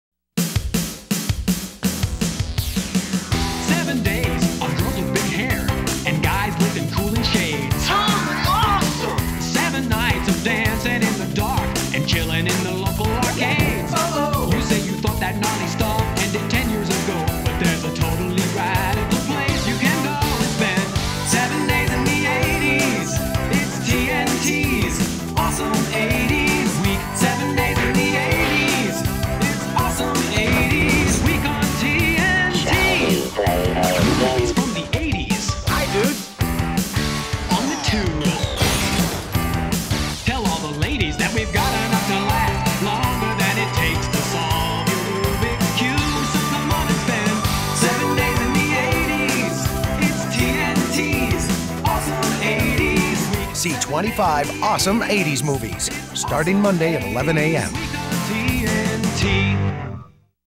Awesome 80's Week
New wave invitation to spend "7 Days in the 80's" with every flick you watched a thousand times on that new life-changing service, Cable TV.  Vocals by me.